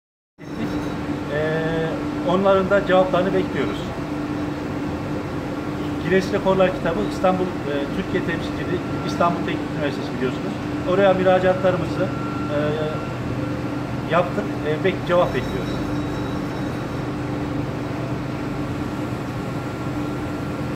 Mustafa Fidanboy
Karaman'ın önemli süt ürünleri üreticilerinden Fidan Süt dünyanın en büyük tulum peynirini üretti.